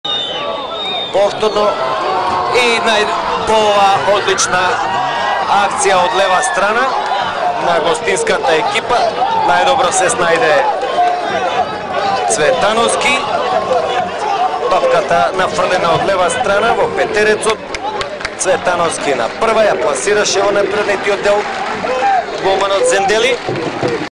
POBEDA SHENON EDHE GOLIN E DYTE NDAJ SHKUPIT (07.05.2017)
Cvetanovski realizoi golin e dyte per Pobeden ne transferten ne Çair.